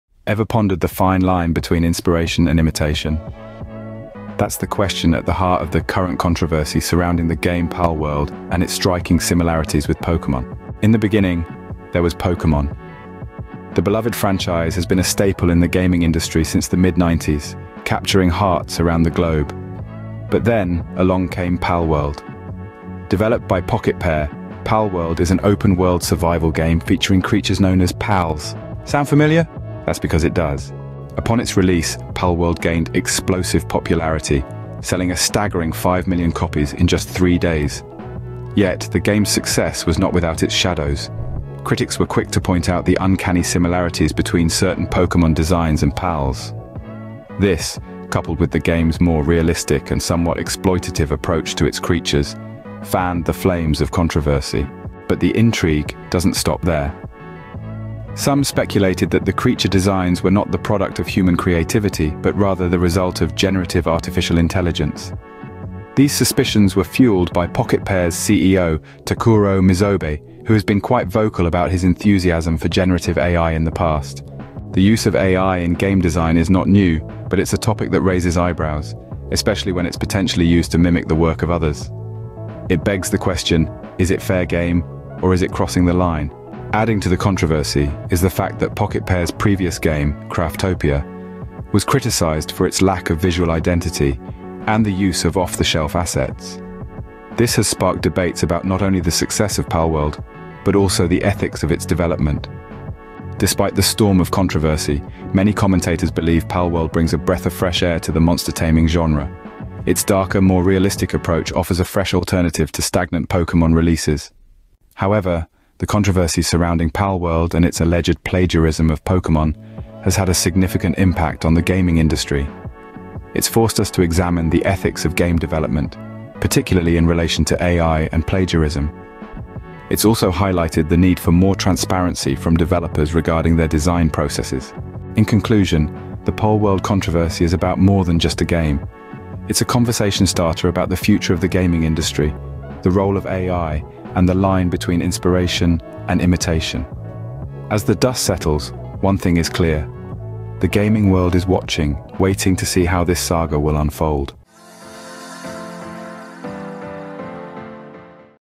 Palworld AI controversy, Pokémon plagiarism accusations explained
Palworld is a new open-world survival game that lets you collect, breed, and fight with creatures called Pals. However, the game has also sparked a lot of controversy over its alleged use of AI to generate its content and its similarities to Pokémon. In this video, we will explain the origins and details of these controversies, and how the developer Pocketpair has responded to them.

00:00:00   The Controversy Begins
00:00:10   Palworld's Introduction
00:00:30   Explosive Popularity
00:00:54   AI Speculations
00:01:14   Ethics of AI in Game Design
00:01:26   Previous Criticisms
00:01:43   Palworld's Fresh Approach
00:01:54   Impact on the Gaming Industry
00:02:15   The Conversation Continues